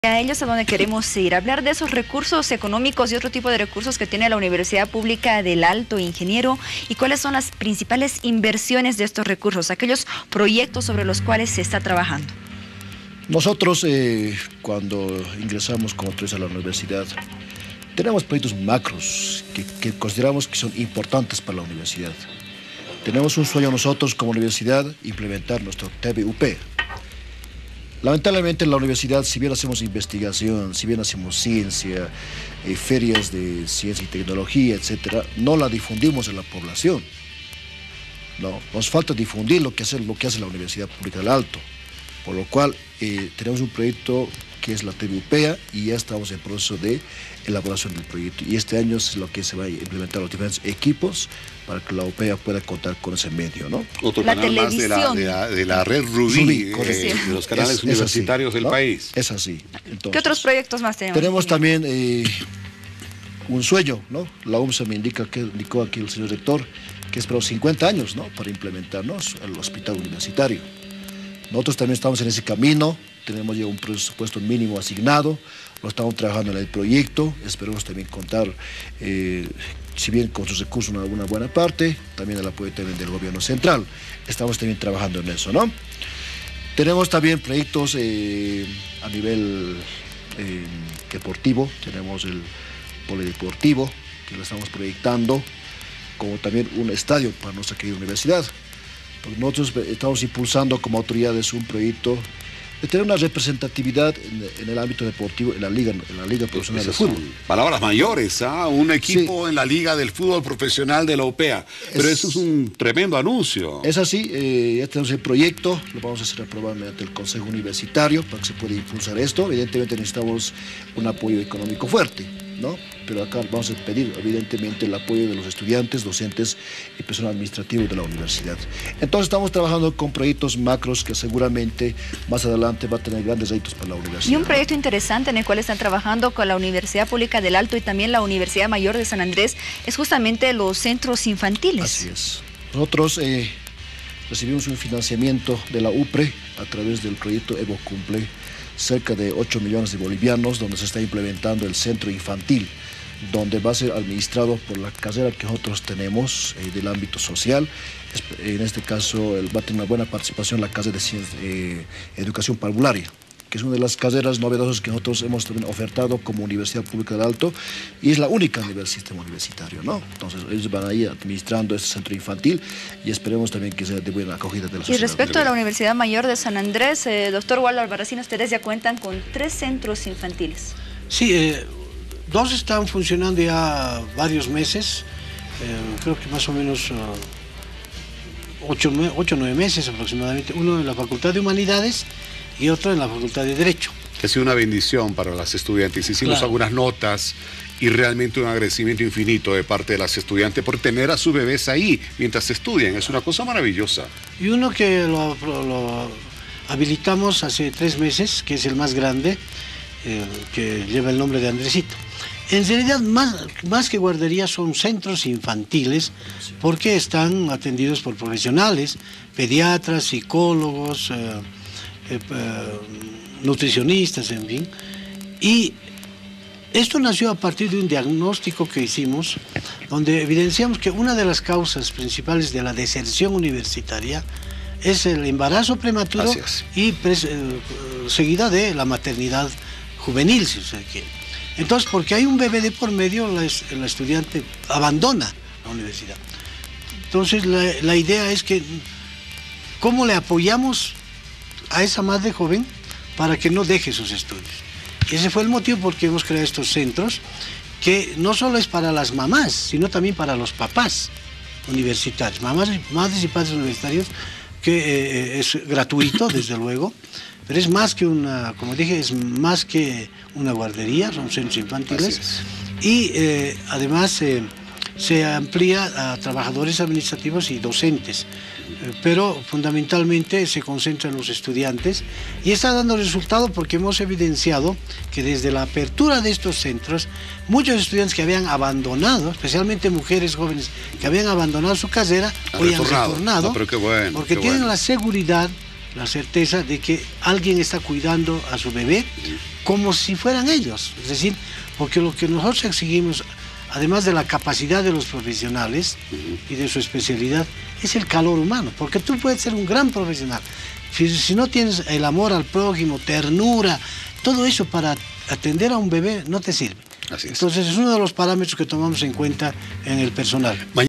Rector de la Universidad Pública de El Alto cuenta los proyectos en los que trabajan
La Universidad Pública de El Alto, trabaja en una serie de proyectos que según cuenta su Rector, hace falta difusión entre la población, desde un centro asistencial, hasta un estadio deportivo, la inversión de los recursos busca fomentar distintas áreas de la educación.

El gasto se realiza en acciones como la investigación